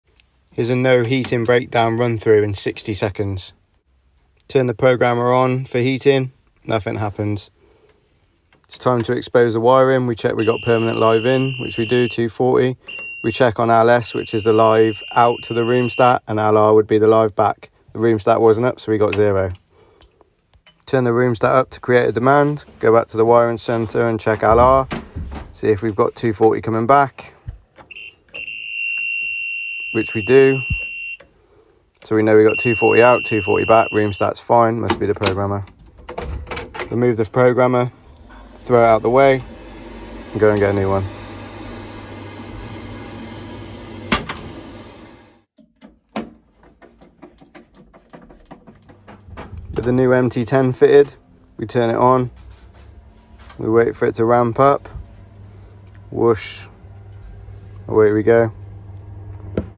60 second no heating breakdown
Called to a no heating breakdown on a Worcester Bosch combi boiler, here’s the diagnosis process explained in 60 seconds